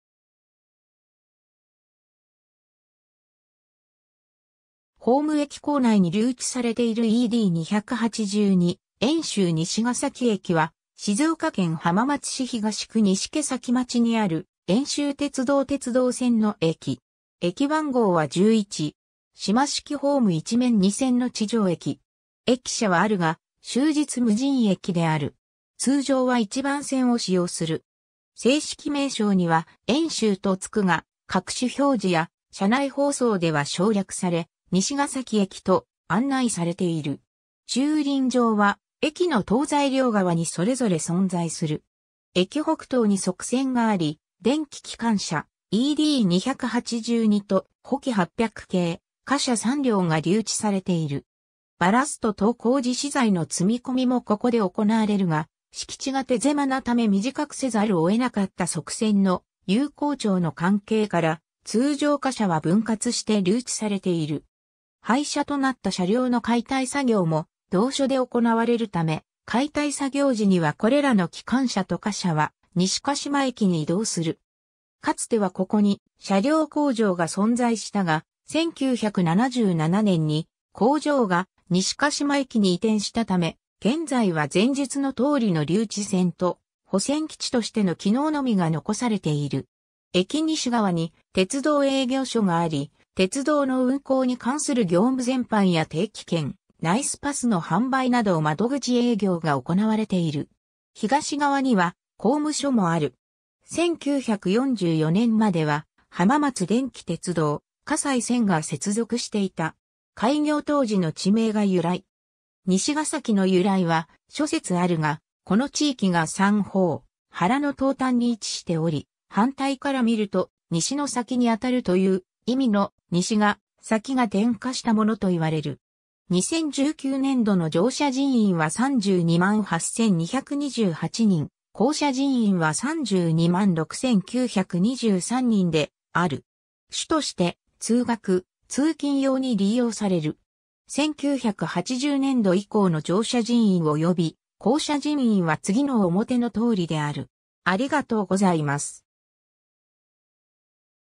遠州西ヶ崎駅
ホーム（2011年1月） 駅構内に留置されている ED282 遠州西ヶ崎駅（えんしゅうにしがさきえき）は、静岡県浜松市東区西ケ崎町にある遠州鉄道鉄道線の駅。
 島式ホーム1面2線の地上駅。
駅舎はあるが、終日無人駅である。
通常は1番線（西側）を使用する。
 正式名称には「遠州」とつくが、各種表示や車内放送では省略され、「西ヶ崎駅」と案内されている。
 駐輪場（収容346台）は駅の東西両側にそれぞれ存在する。
 駅北東に側線があり、電気機関車ED282と、ホキ800形貨車3両が留置されている。
バラスト等工事資材の積み込みもここで行われるが、敷地が手狭なため短くせざるを得なかった側線の有効長の関係から、通常貨車は分割して留置されている。
廃車となった車両の解体作業も同所で行われるため、解体作業時にはこれらの機関車と貨車は西鹿島駅に移動する。
かつてはここに車両工場が存在したが、1977年（昭和52年）に工場が西鹿島駅に移転したため、現在は前述の通りの留置線と保線基地としての機能のみが残されている。
 駅西側に鉄道営業所があり、鉄道の運行に関する業務全般や定期券・ナイスパスの販売など窓口営業が行われている。
東側には工務所もある。
 1944年（昭和19年）までは、浜松電気鉄道笠井線が接続していた。
 開業当時の地名（浜名郡積志村大字西ヶ崎）が由来。
 「西ヶ崎」の由来は諸説あるが、この地域が三方原の東端に位置しており「反対から見ると西の先にあたる」と言う意味の「西ヶ先」が転化したものといわれる。
 2019年度（令和元年度）の乗車人員は328,228人（全18駅中16位）、降車人員は326,923人（同16位）である。
主として通学・通勤用に利用される。
 1980年度（昭和55年度）以降の乗車人員および降車人員は次の表のとおりである。